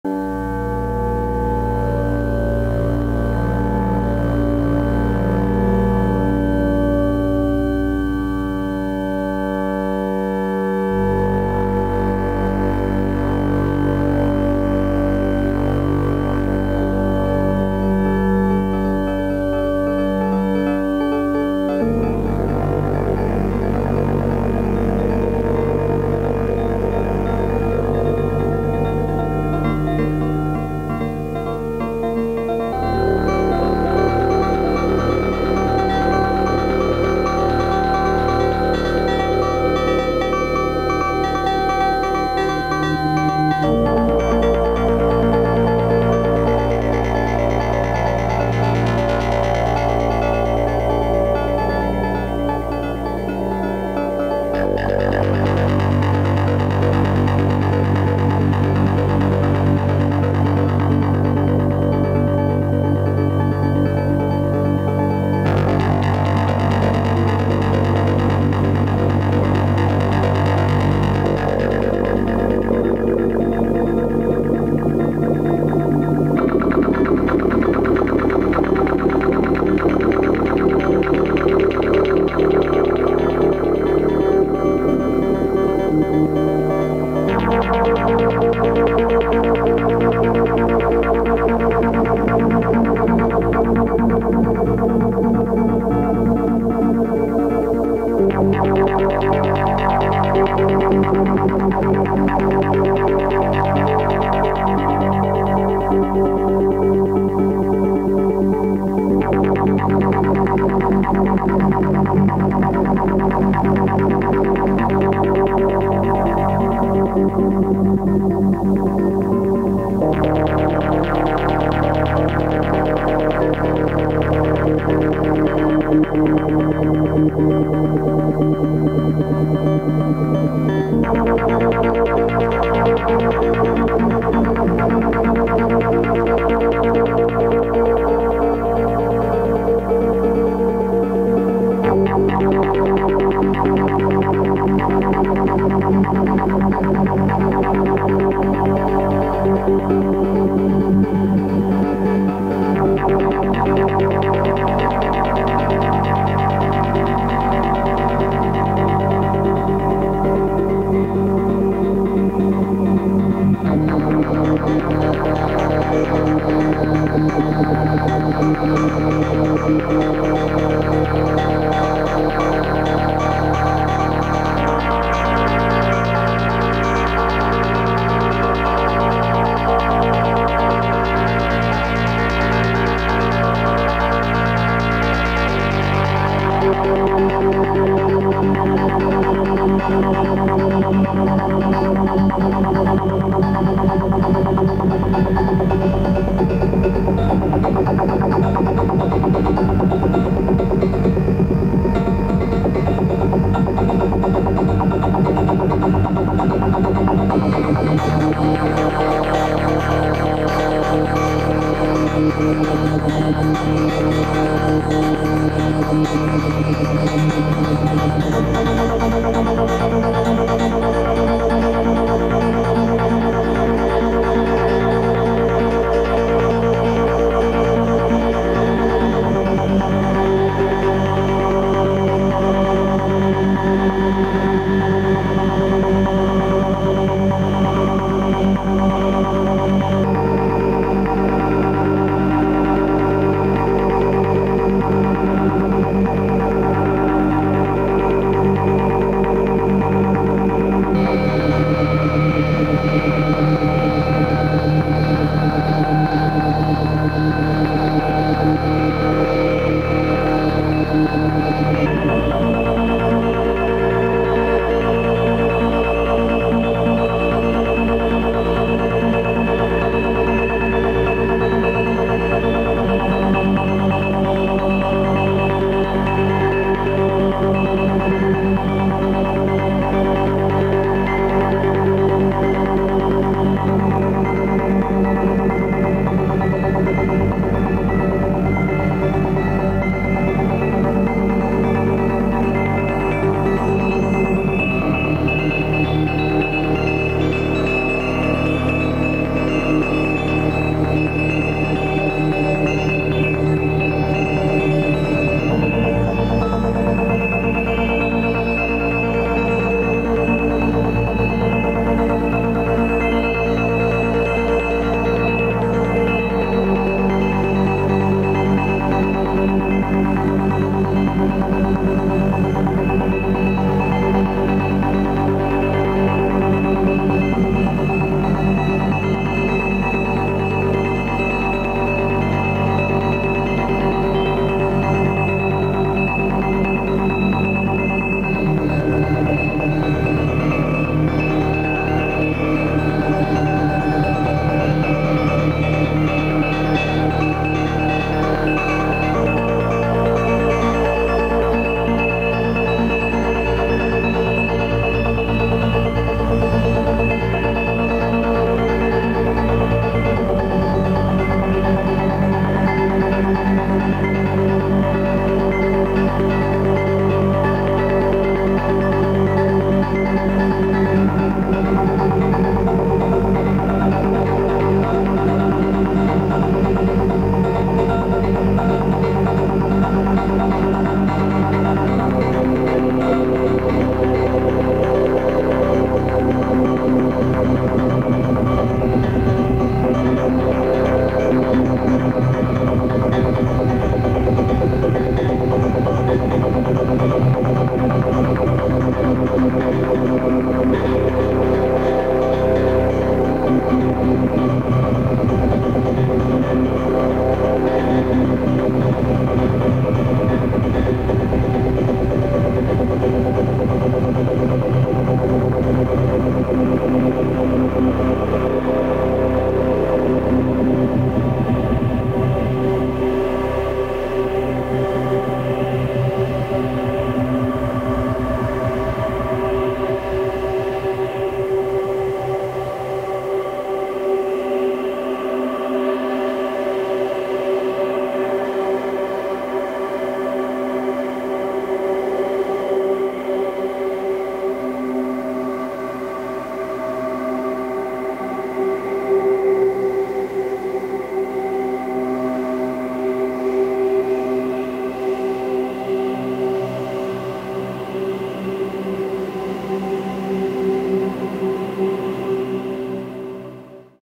Baker At Shuksan
Video from Mt. Baker National Forest, WA
Eurorack modular synth live performance featuring PNW, Marbles, Plaits, MCO, Rings, WASP, Optomix, Mimeophon.